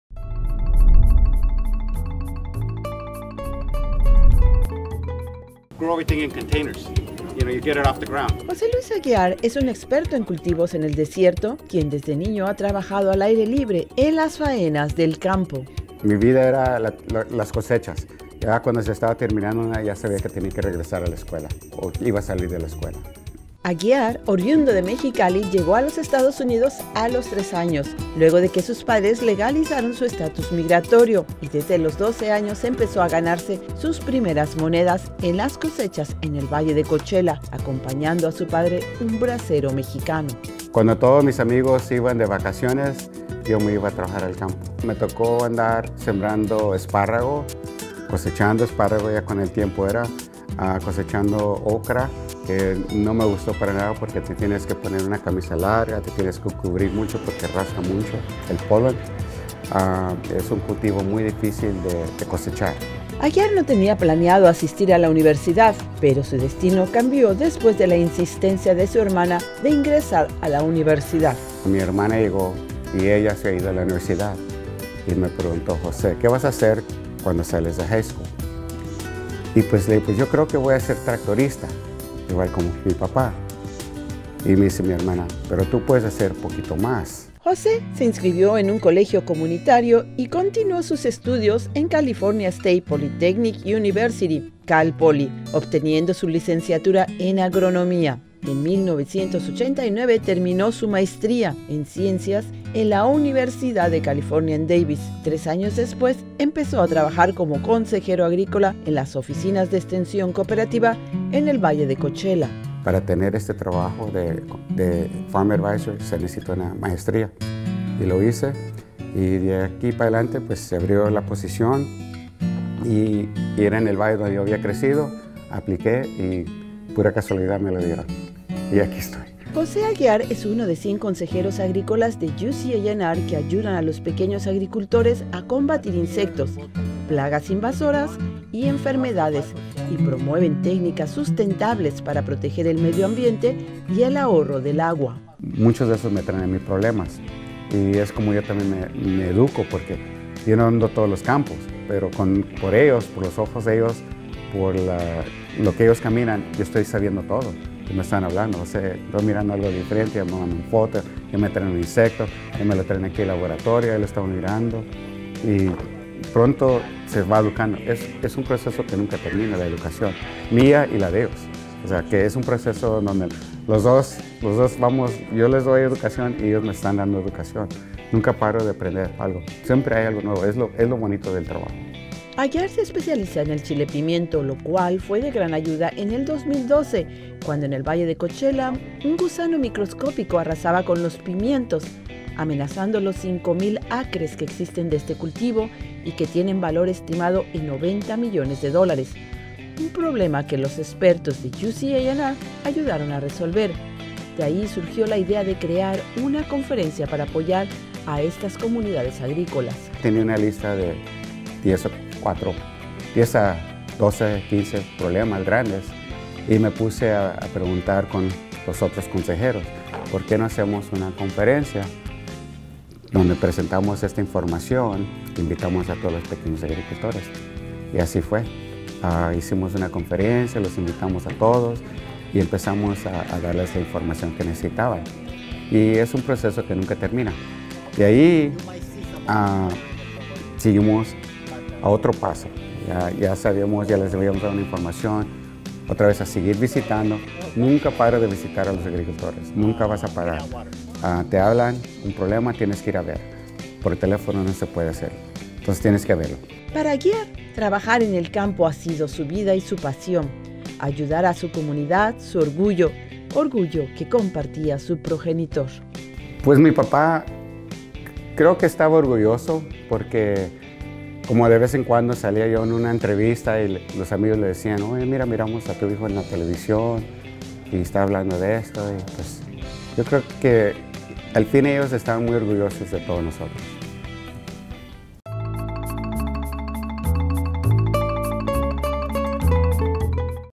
José L. Aguiar - De niño campesino a experto agrícola
UC ANR celebra el Mes de la Hispanidad presentando la vida y las contribuciones a la comunidad de California de cinco Latinos expertos agrícolas y educadores de nutrición de los diferentes programas de la División de Agricultura y Recursos Naturales de la Universidad de California. presentamos el perfil de José Luis Aguiar, consejero agrícola de UC ANR en el área de Coachella. Aguiar tiene 26 años en la investigación agrícola, y se ha convertido en una persona indispensable para muchos agricultores de su área, ellos aprecian su trabajo, al tiempo que sus padres admiran su perseverancia.